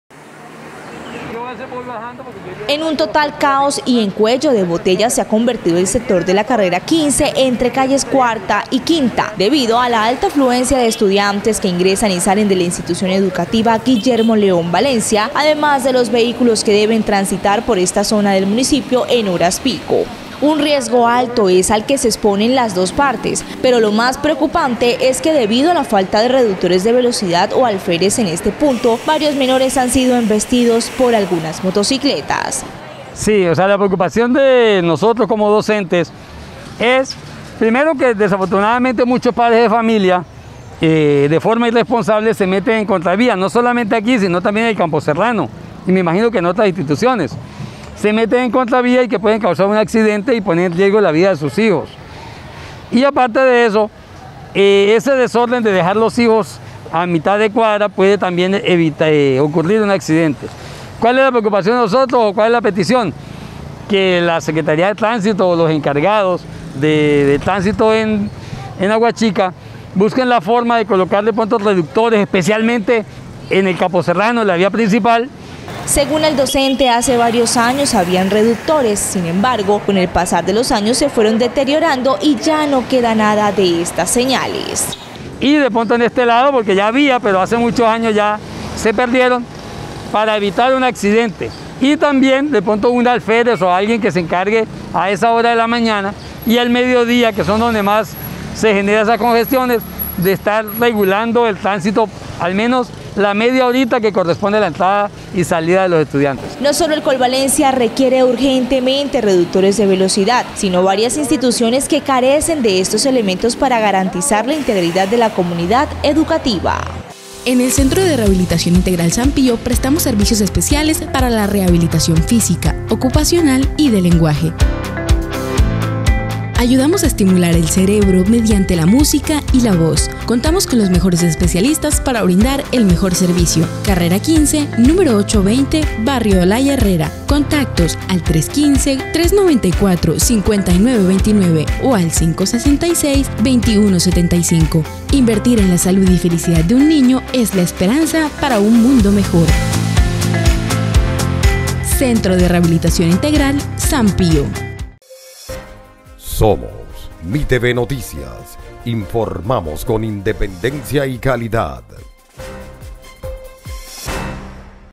CAOS VIAL EN LA CRA15 POR FALTA DE SEÑALIZACIÓN
Caos vial en la carrera 15 por falta de señalización, estudiantes han resultado heridos por el flujo vehicular.